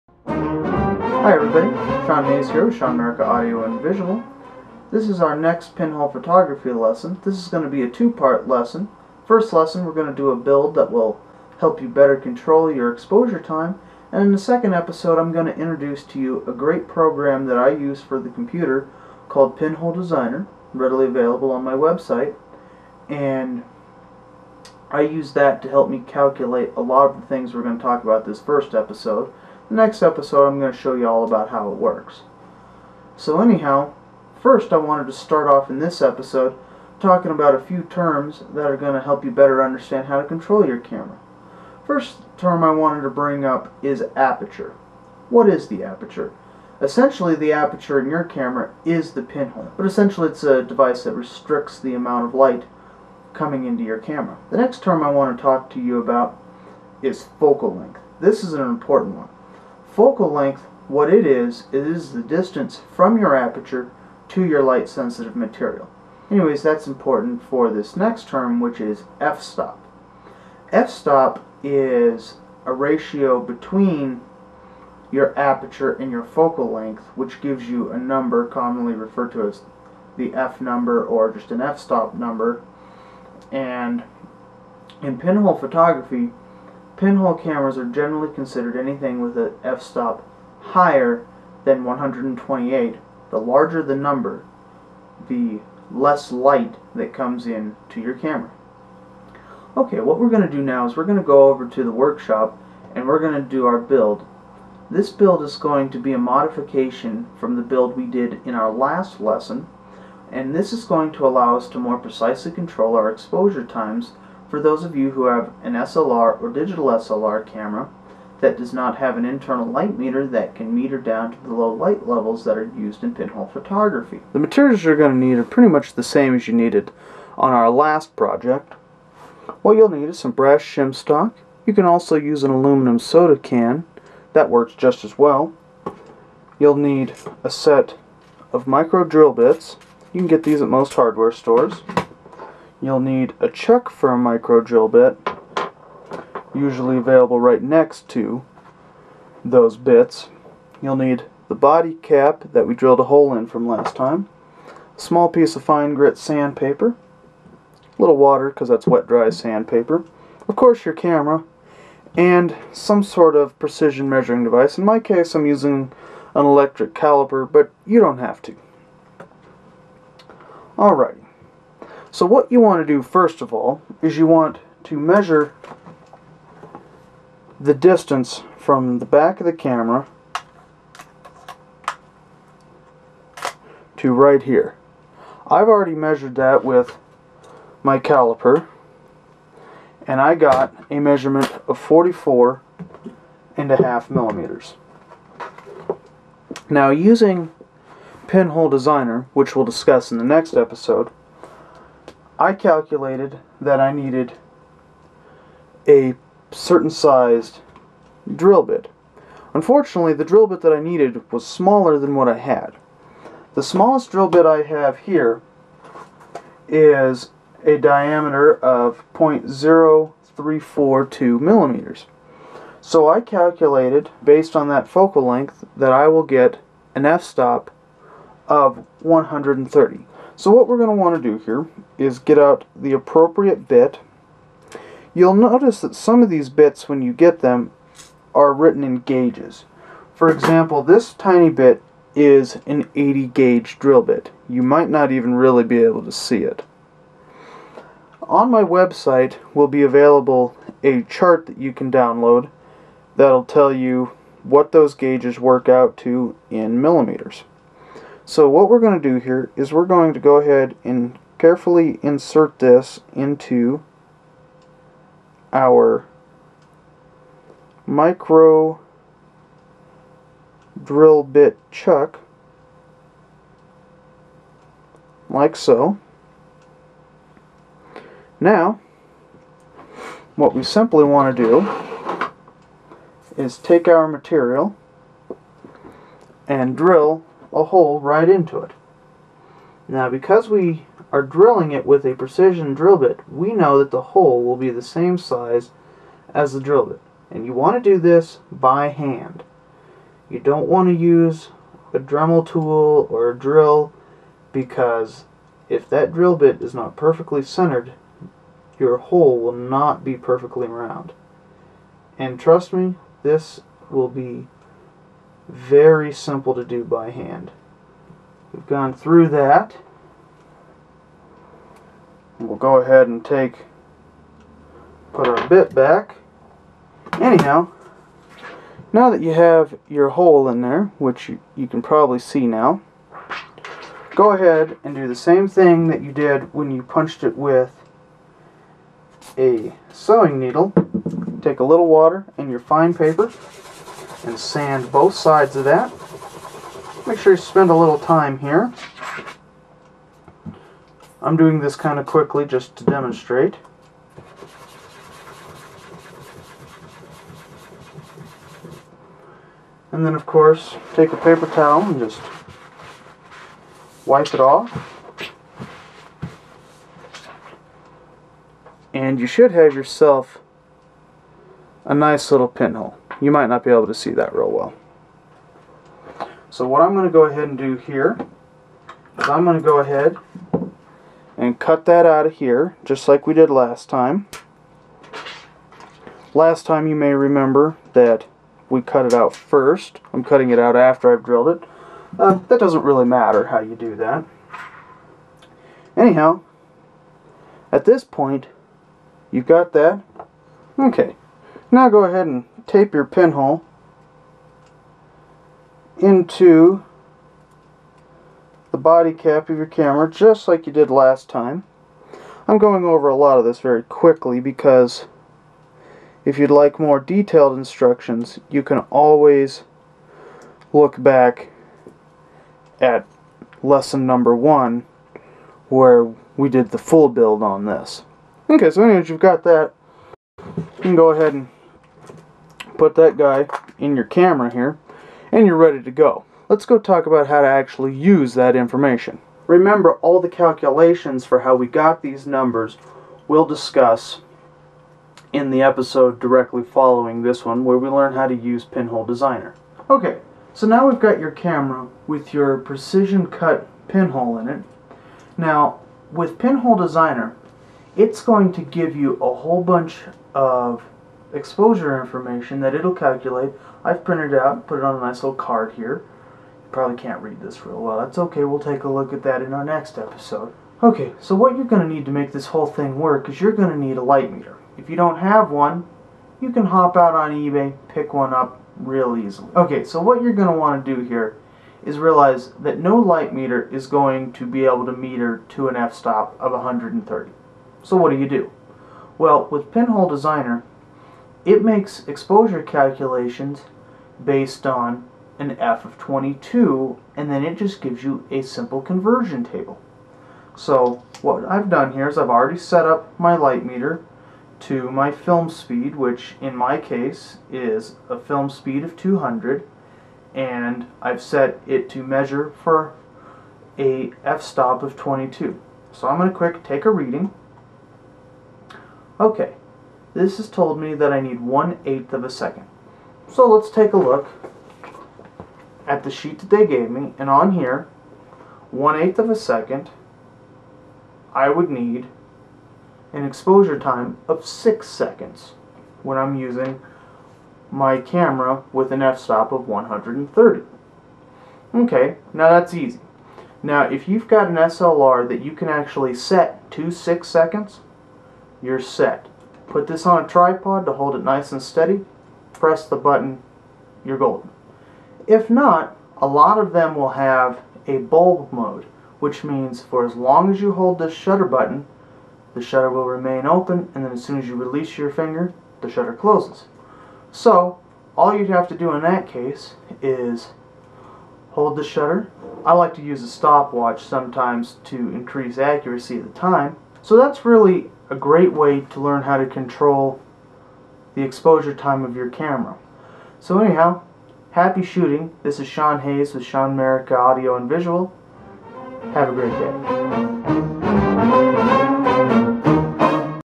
How to Pinhole Camera Photography (Lesson 2, 1of 2) 002
In this lesson we are going to take a look at how to better control your exposure time.  This is part one of a two part lesson. Remember to have fun. Make sure to visit all of my lessons about pinhole cameras and pinhole photography.